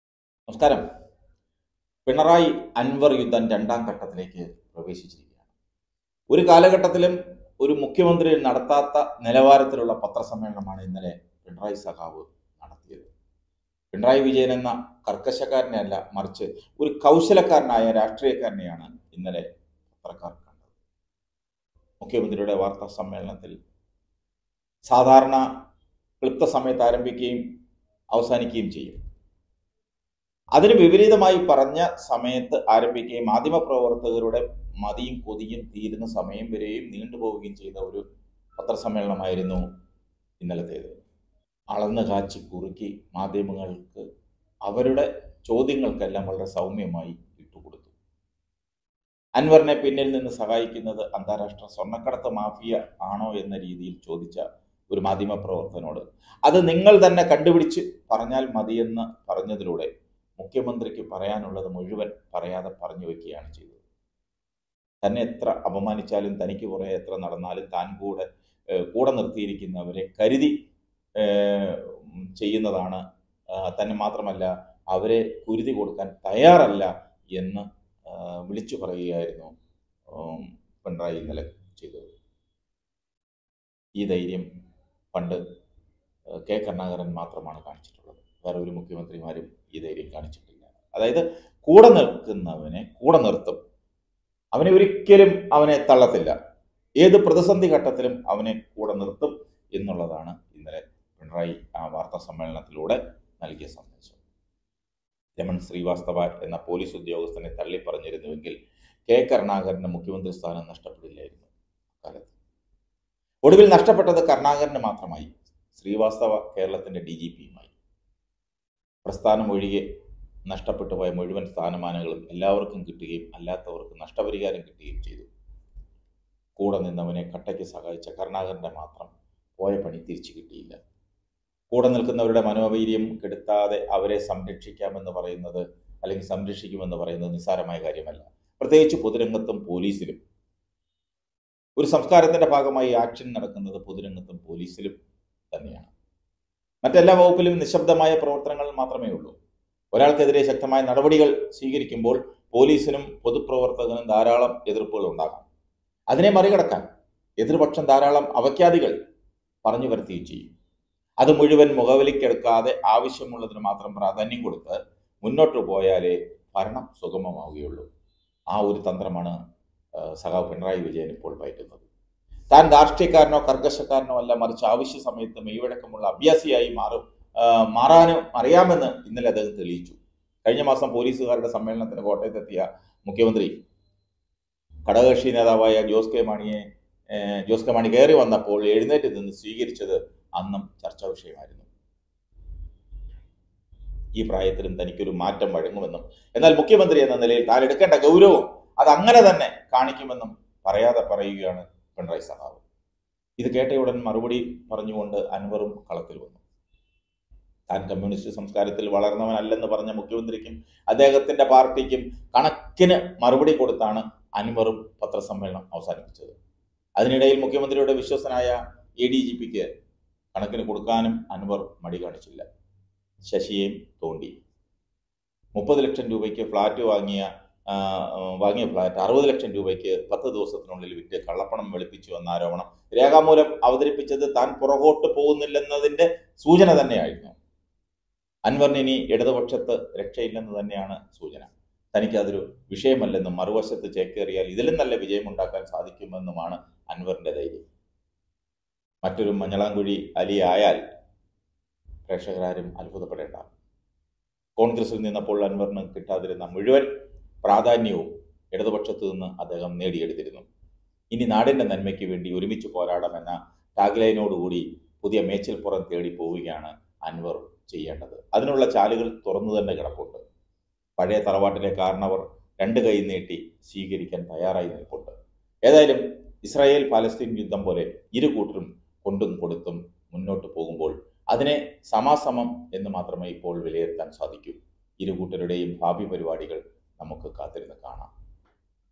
PINARAYI VIJAYAN |അൻവറിനെ പുറത്താക്കാൻ പിണറായി സഖാവിന്റെ ഉരുള് നേർച്ച , നിലമ്പൂർ CPMവെട്ടിൽ |CPM|LDF
അൻവറിനെ പൂട്ടാൻ പിണറായി സഖാവിന്റെ തിരക്കഥ , തെളിവ് നൽകിയില്ലെങ്കിൽ നടപടി